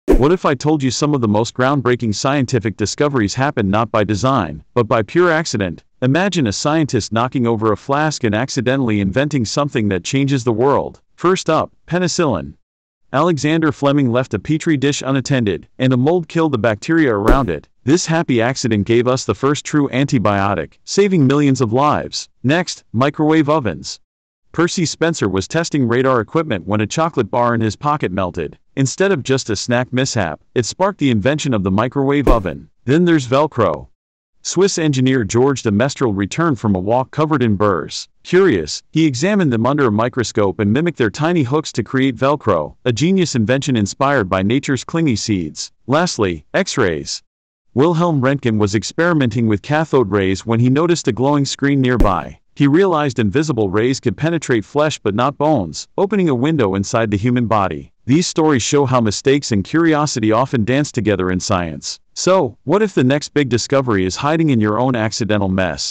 4 Incredible Inventions Discovered by Pure Accident!
Discover how some of the world's most groundbreaking inventions—like penicillin, microwave ovens, Velcro, and X-rays—were created by unexpected accidents and curious minds. This video reveals the fascinating stories behind these life-changing discoveries and shows how mistakes can lead to scientific breakthroughs. Watch now to get inspired by the power of curiosity and happy accidents!